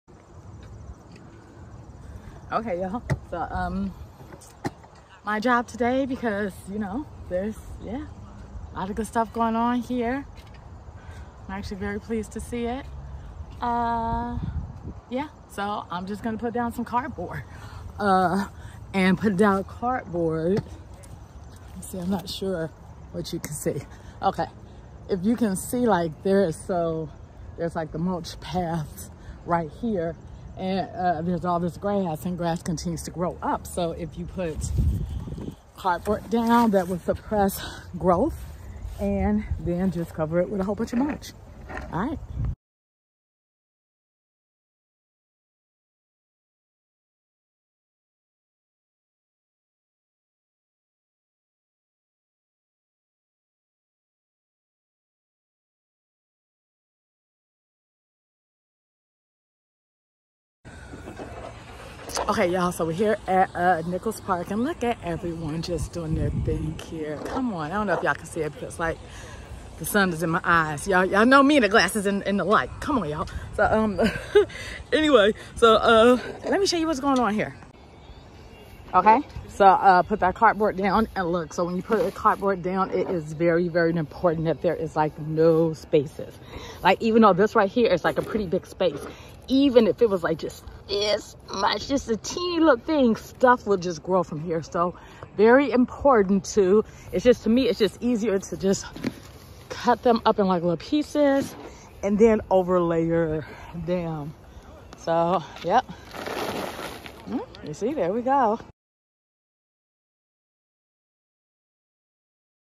How to kill grass and weeds with cardboard for garden
See how you can naturally kill grass and weeds around garden beds with cardboard for your vegetable garden. I have been using cardboard as an organic way to kill grass and plant my garden.

Get your FREE Fall Planting Garden Guide!

Reach your highest vibration.